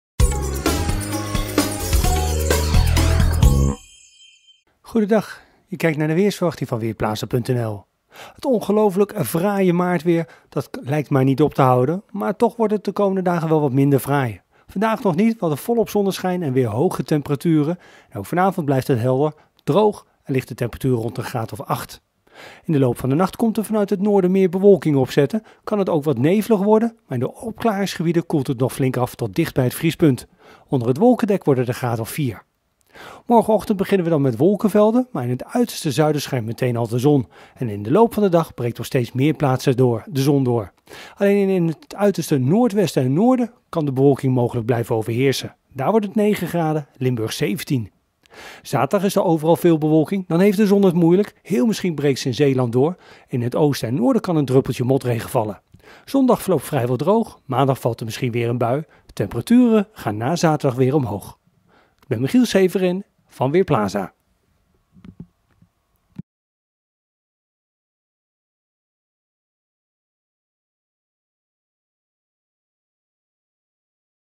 Weerbericht donderdagmiddag: Vrijdag meer wolken
In de loop van de nacht neemt vanuit het noordwesten en noorden de bewolking toe. Onder het wolkendek is het 4 graden, bij brede opklaringen rond het vriespunt en soms mistig. Morgen overdag lost de bewolking vanuit het zuiden op en schijnt de zon weer flink. Het wordt 9 graden langs de noordkust en 17 in Limburg. Zaterdag is het overwegend bewolkt met kans op lichte regen. Bij een stevige wind wordt het 8 tot 11 graden. Zondag breekt de zon weer door.